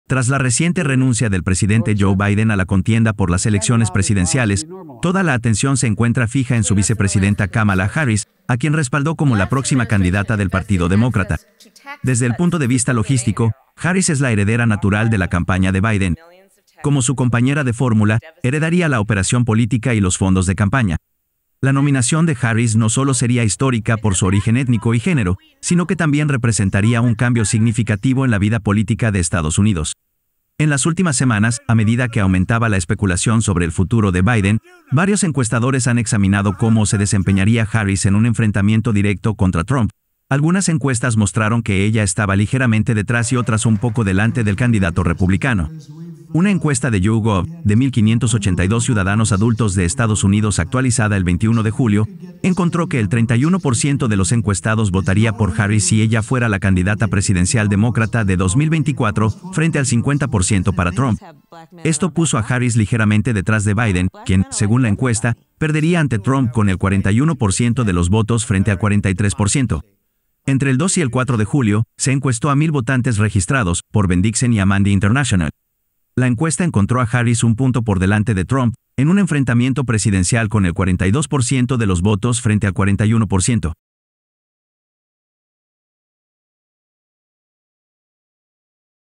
¿Kamala Harris podría vencer a Trump?
Tras la reciente renuncia del presidente Joe Biden a la contienda por las elecciones presidenciales, toda la atención se encuentra fija en su vicepresidenta Kamala Harris, a quien respaldó como la próxima candidata del partido demócrata.